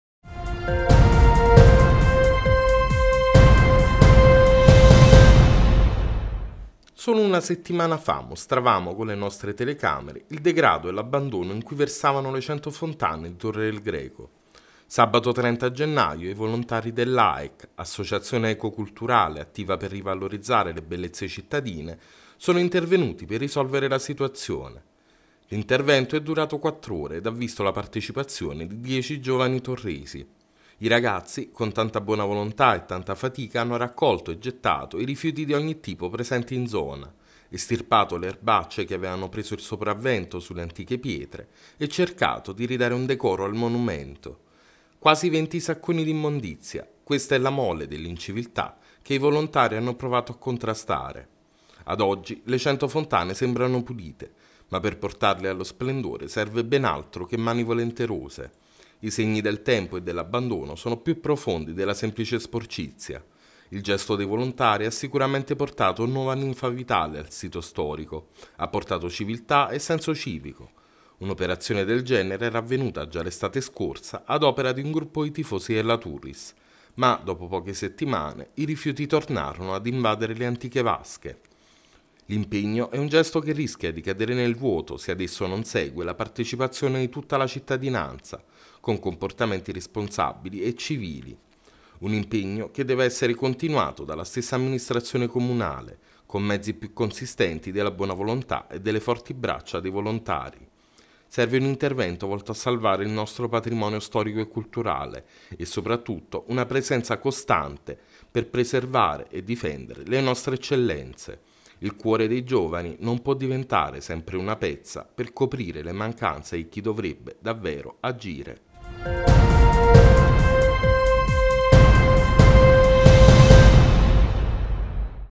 Volontari dell'AEC bonificano le 100 Fontane di Torre del Greco - VIDEO
Solo una settimana fa mostravamo, con le nostre telecamere, il degrado e l’abbandono in cui versavano le 100 Fontane di Torre del Greco. Sabato 30 gennaio i volontari dell’A.E.C., associazione eco culturale attiva per rivalorizzare le bellezze cittadine, sono intervenuti per risolvere la situazione. L’intervento è durato 4 ore ed ha visto la partecipazione di 10 giovani torresi. I ragazzi, con tanta buona volontà e tanta fatica hanno raccolto e gettato i rifiuti di ogni tipo presenti in zona, estirpato le erbacce che avevano preso il sopravvento sulle antiche pietre e cercato di ridare un decoro al monumento. Quasi venti sacconi di immondizia, questa è la mole dell’inciviltà che i volontari hanno provato a contrastare.
Ad oggi, le 100 fontane sembrano pulite, ma per portarle allo splendore serve ben altro che mani volenterose. I segni del tempo e dell’abbandono sono più profondi della semplice sporcizia. Il gesto dei volontari ha sicuramente portato nuova ninfa vitale al sito storico, ha portato civiltà e senso civico.
Un’operazione del genere era avvenuta già l’estate scorsa, ad opera di un gruppo di tifosi della Turris, ma, dopo poche settimane, i rifiuti tornarono ad invadere le antiche vasche. L’impegno è un gesto che rischia di cadere nel vuoto se ad esso non segue la partecipazione di tutta la cittadinanza, con comportamenti responsabili e civili. Un impegno che deve essere continuato dalla stessa amministrazione comunale, con mezzi più consistenti della buona volontà e le forti braccia dei volontari. Serve un intervento volto a salvare il nostro patrimonio storico e culturale e, soprattutto, una presenza costante volta a preservare e difendere le nostre eccellenze. Il cuore dei giovani non può diventare sempre una pezza per coprire le mancanze di chi dovrebbe, davvero, agire.